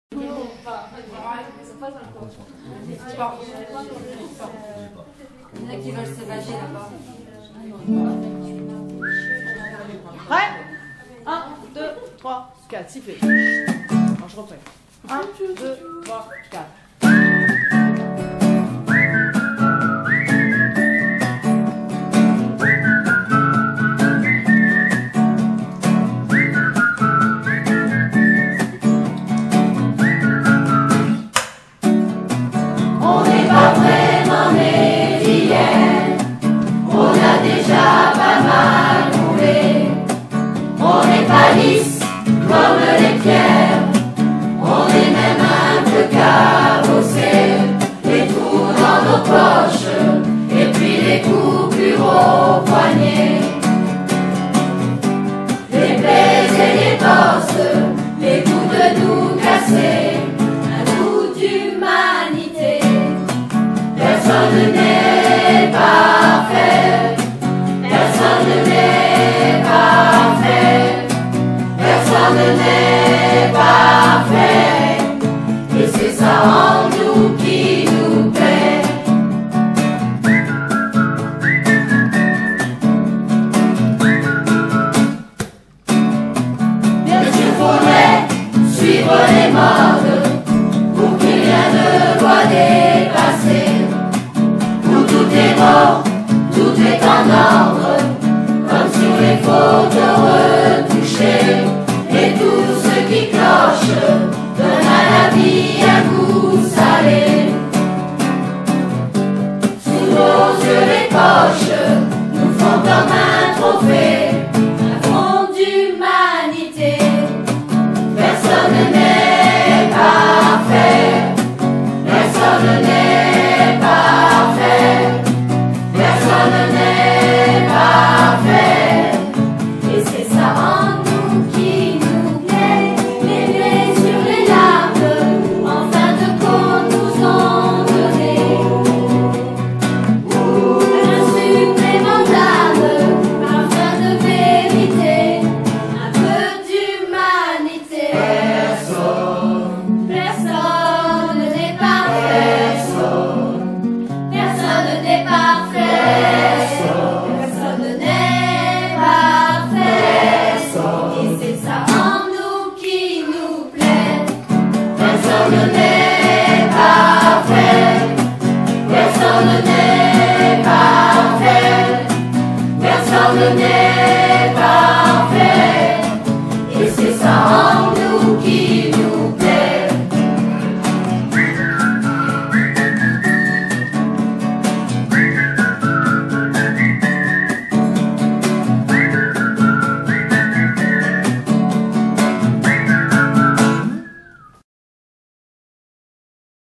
la sacrée chorale répète
La Sacrée Chorale a deux ans d'existence et a été créée par Cécile Perfetti, animatrice musique à la Maison des Arts intercommunale de Nègrepelisse.
 Cet ensemble vocal amateur a la particularité de réunir autour du chant des personnes qui ne se seraient sans doute jamais rencontrées: des Grand-Mères, des jeunes ados, des enfants qui échangent et mêlent leurs différences avec humilité et bonne humeur...